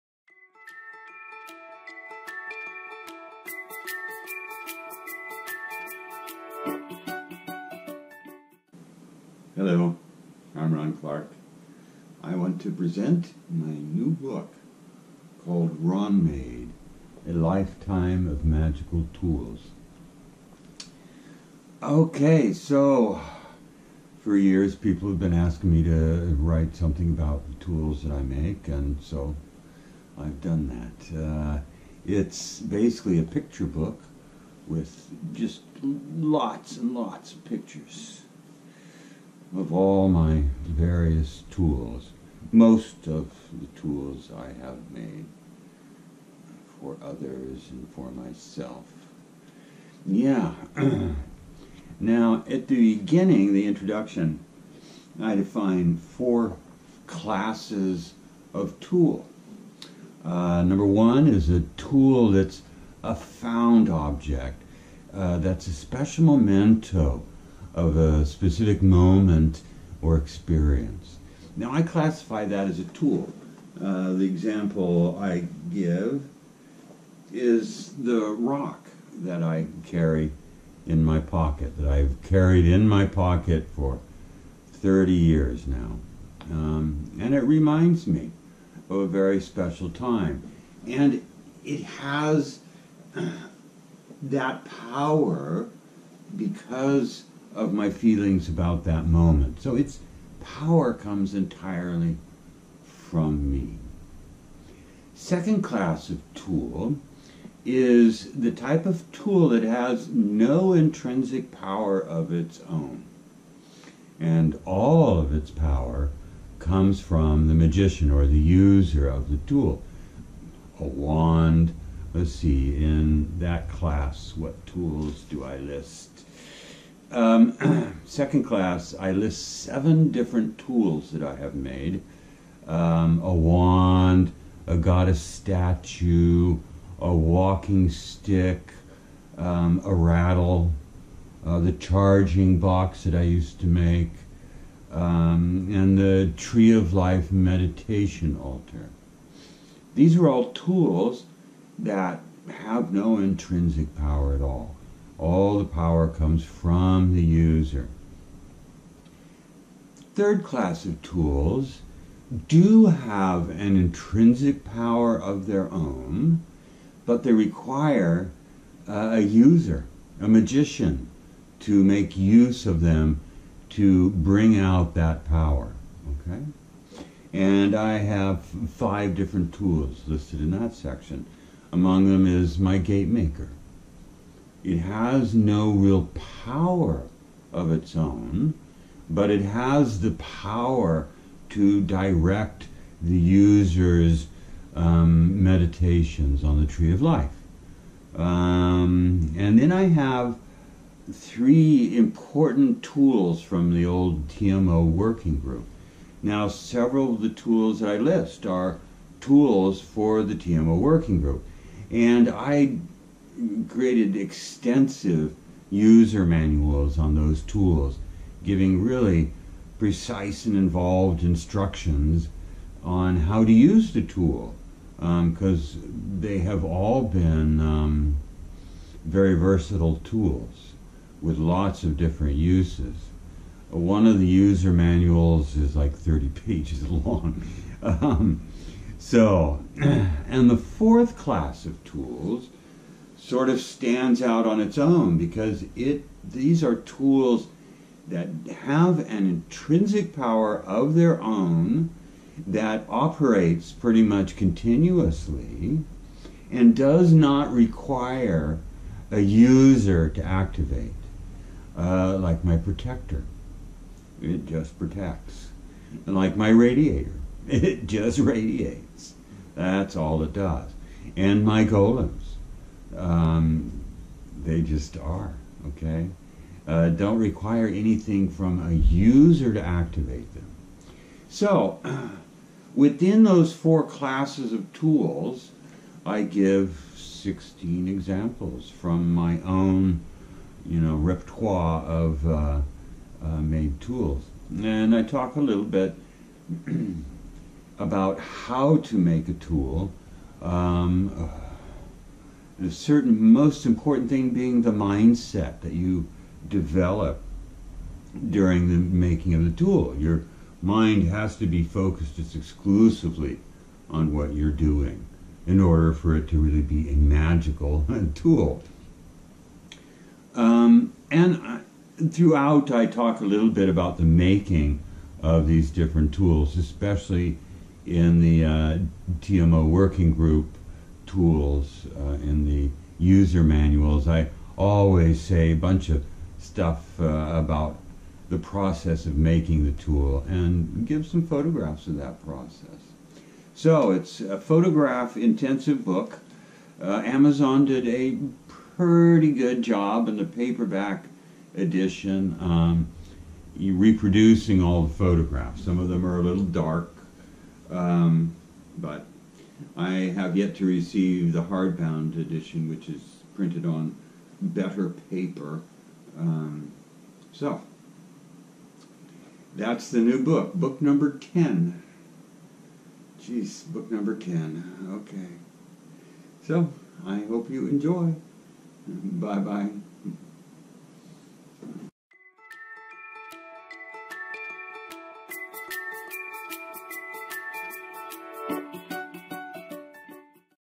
My Books: RawnMade - A Lifetime of Magical Tools
A celebration of my ten published books in which I (Rawn Clark) talk about their origins, publishing history and price.  Today, I present you with my 10th book!

RawnMade was the name of my old website through which I sold magical tools and various pieces of art made with my own hands, so I thought it made the best title for a book about magical tools!  LOL  I give 16+ different examples from the tools I've made over the past 30 years and the book is filled with color pictures of most of them.

As with all my books, this one is available through Amazon

Price is $21.50 for paperback and $38.00 for hardback.